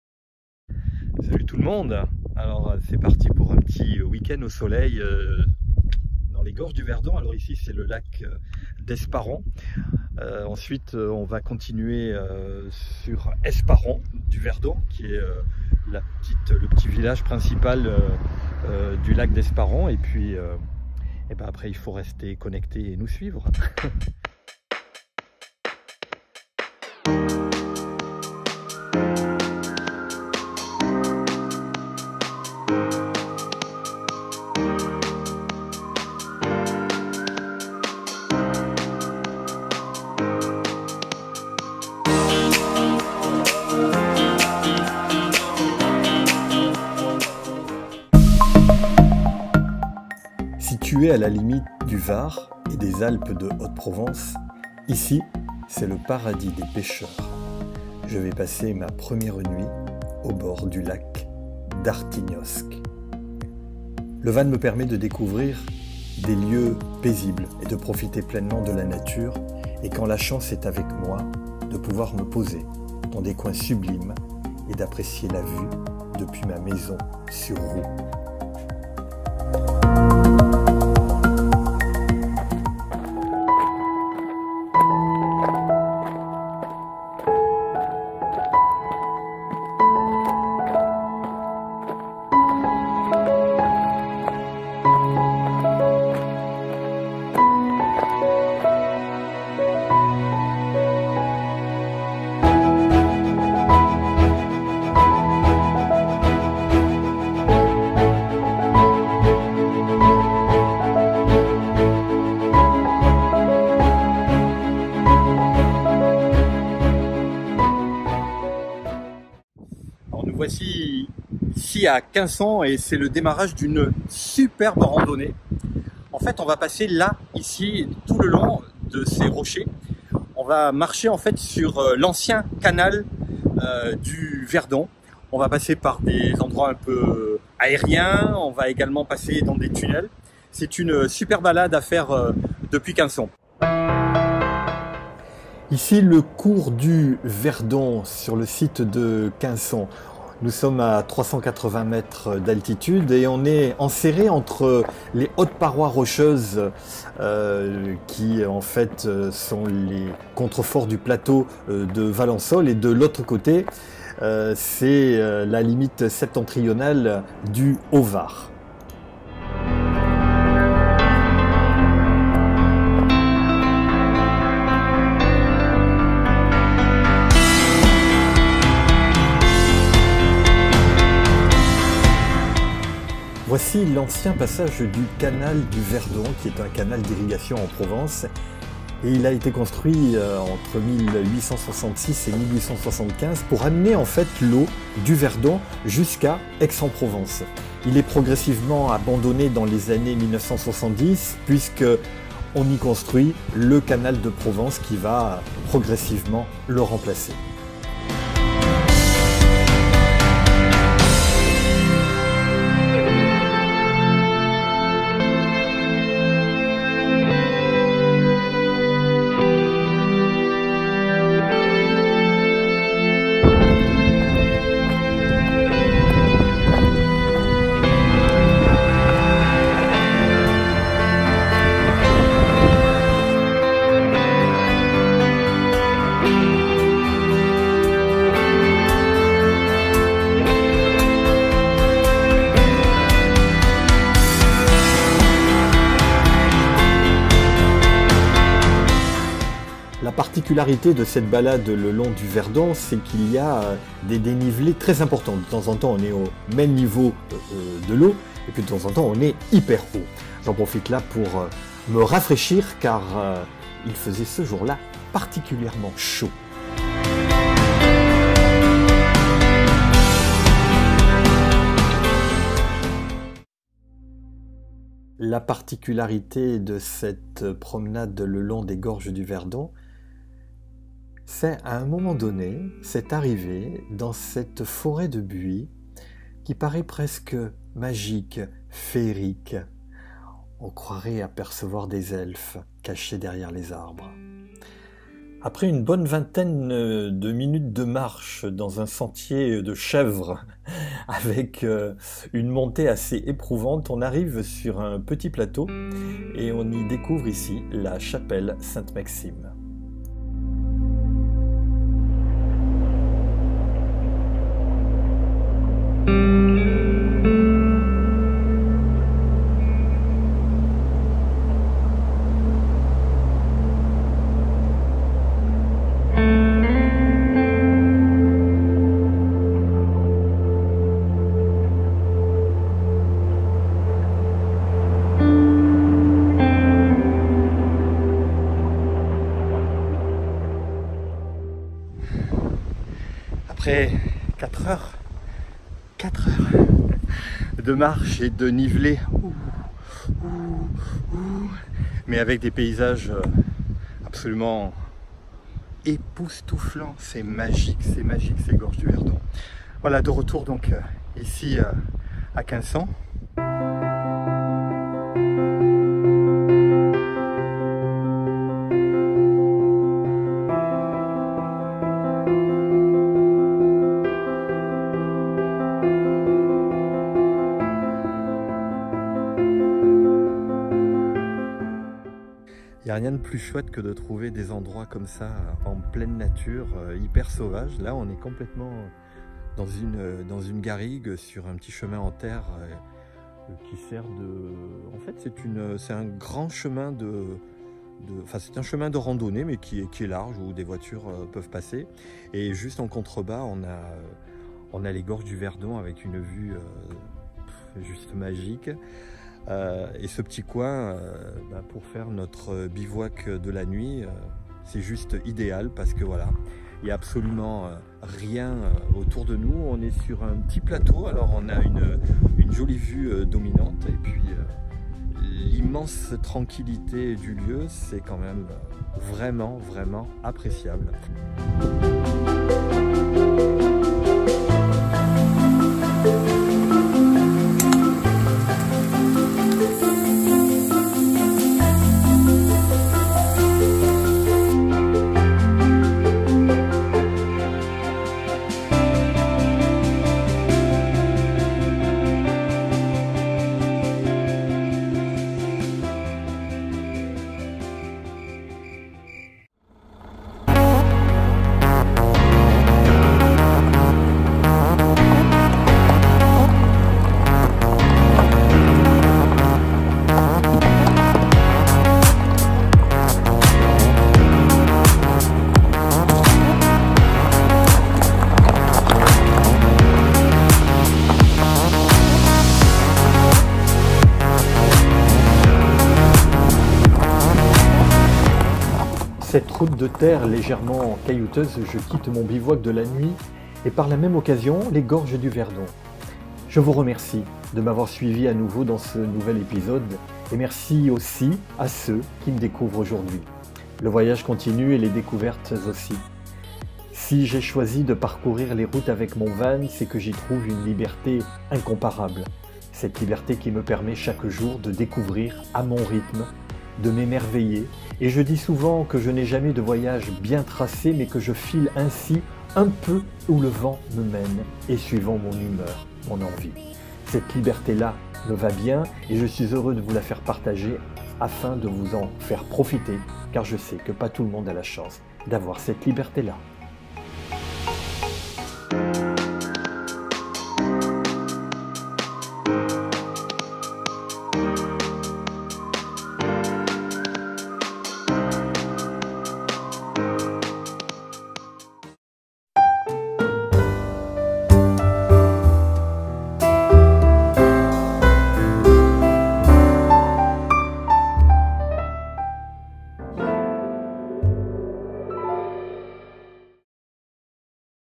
#16 BIVOUAC EN VAN DANS LES GORGES DU VERDON
épisode #16 BIVOUAC EN VAN DANS LES GORGES DU VERDON
du lac d'Esparon au lac de Quinson en passant par les Gorges du Verdon avec randonnée dans l'ancien canal du Verdon. Point de bivouac en van en pleine nature avec vue sur les gorges du Verdon.

Suivez-moi !!!